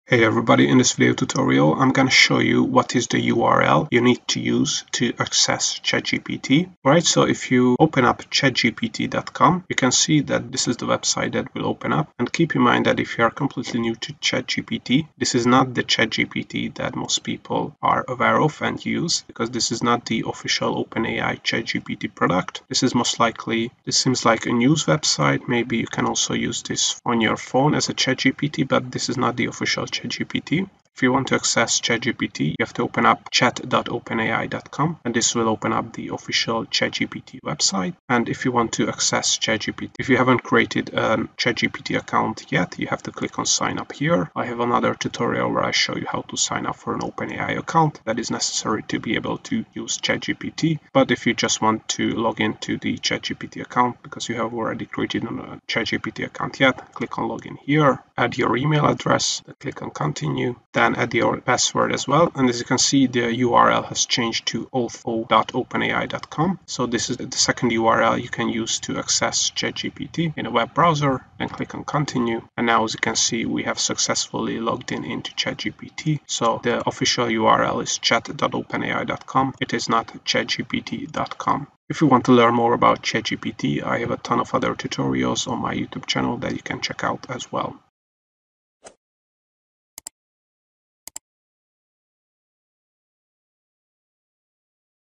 What Is The URL Of ChatGPT?
. - Learn what the ChatGPT URL is that you should use to access ChatGPT.